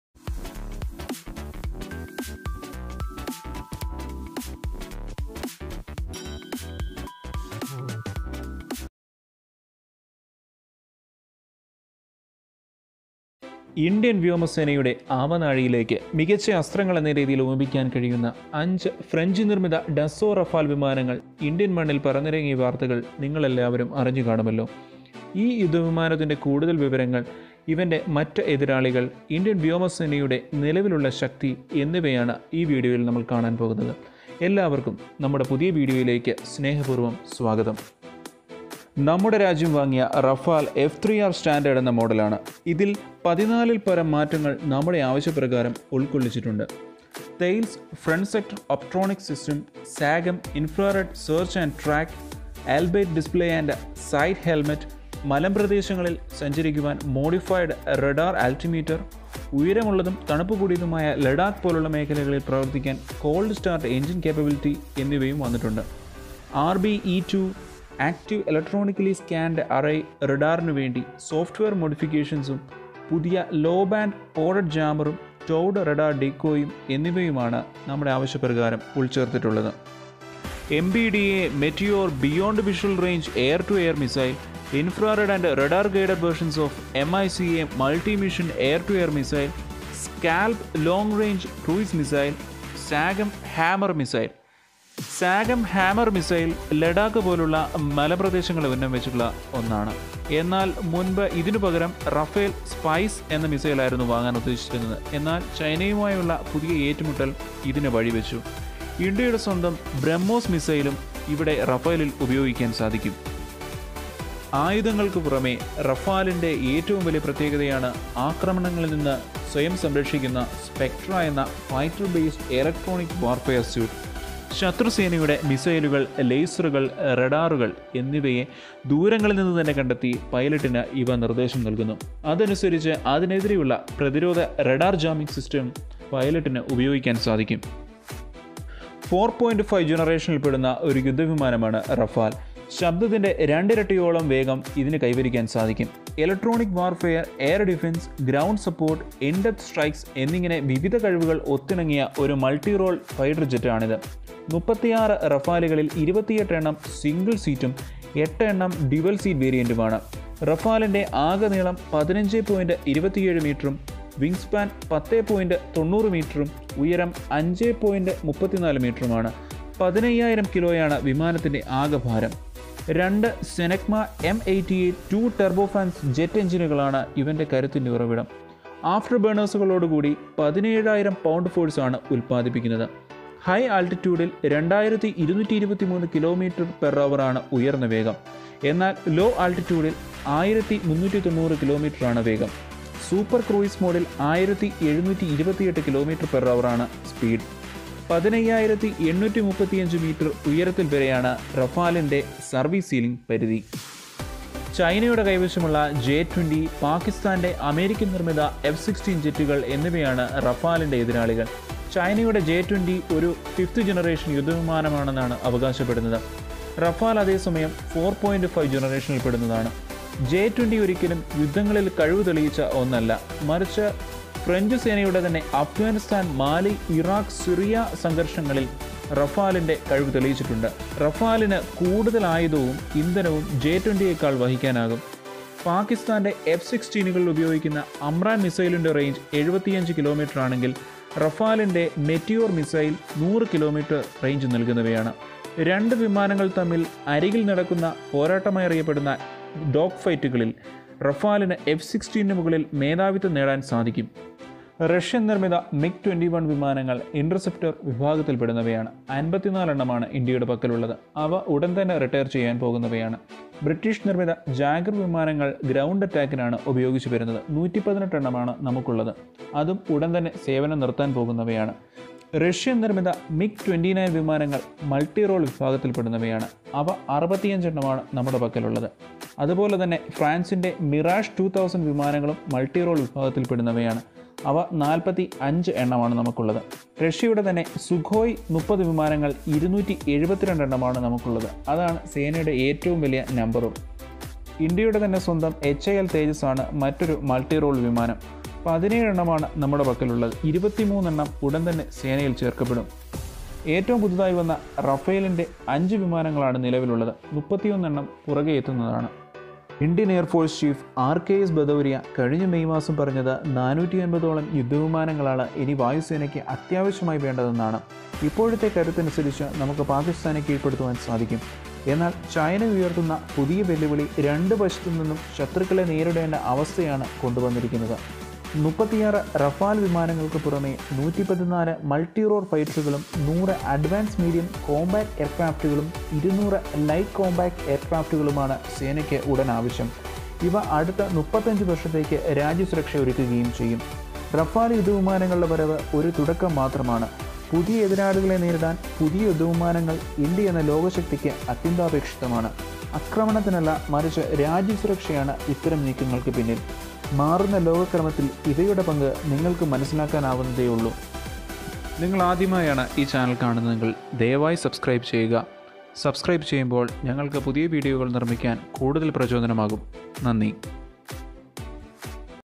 Indian Rafale Fact check video in Malayalam (മലയാളം) | Limone Auto World
The Dassault Rafale is a French twin-engine, canard delta wing, multirole fighter aircraft designed and built by Dassault Aviation. Equipped with a wide range of weapons, the Rafale is intended to perform air supremacy, interdiction, aerial reconnaissance, ground support, in-depth strike, anti-ship strike and nuclear deterrence missions. The Rafale is referred to as an "omnirole" aircraft by Dassault

The French-made Rafales are India's first major acquisition of fighter planes in 23 years after the Sukhoi jets were imported from Russia.

*Credits*

Bharatsakthi.in, Thedrive.com, ndtv.com, wikipedia.in, The Indian Express

Mr. Arun Moozhiplackal -
Former batalion senior under officer
17(K) Bn NCC Pala-Army Wing

Video footages- Thedassault Aviation, Spunky Warriors

Images- Google, Wikipedia